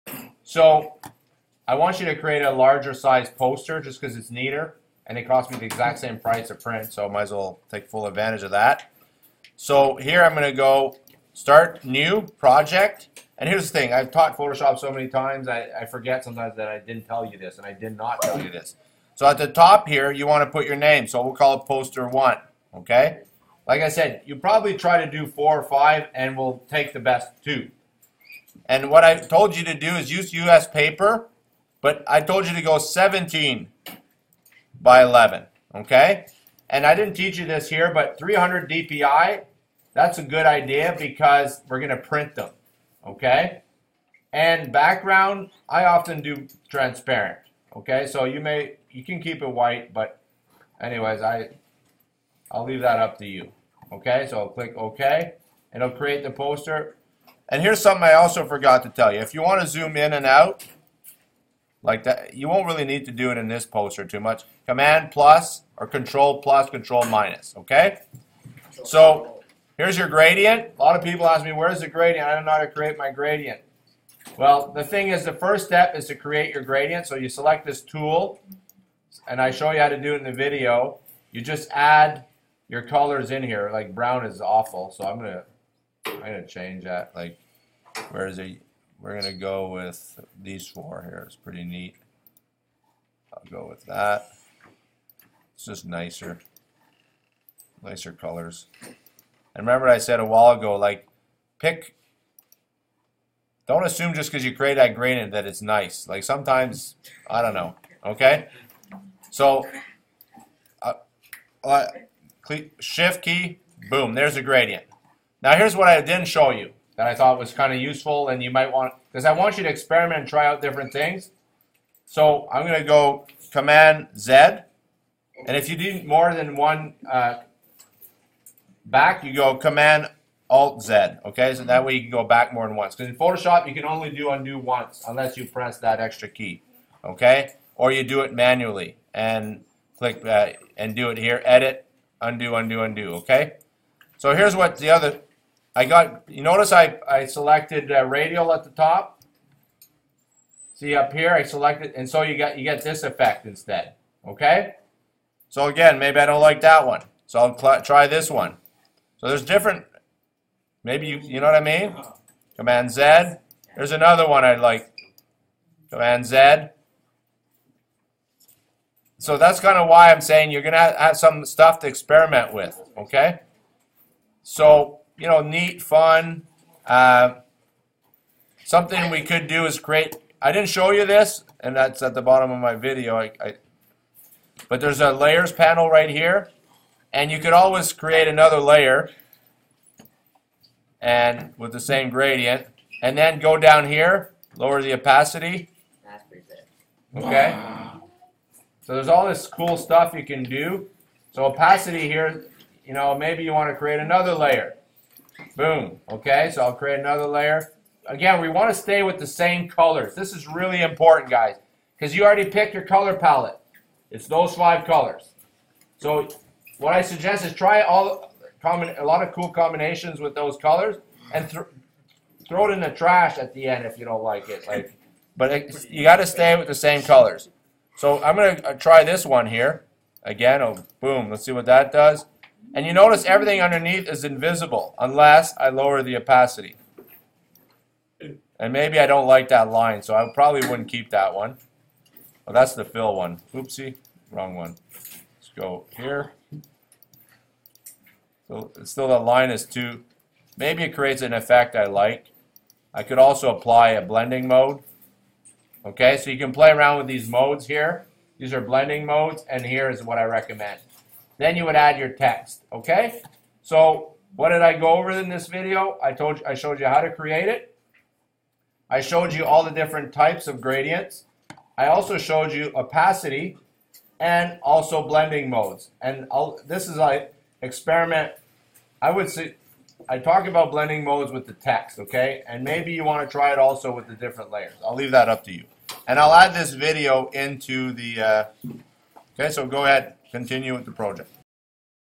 Types of Gradients, and basics of Layers
This video shows you how to use different types of gradients in your classes.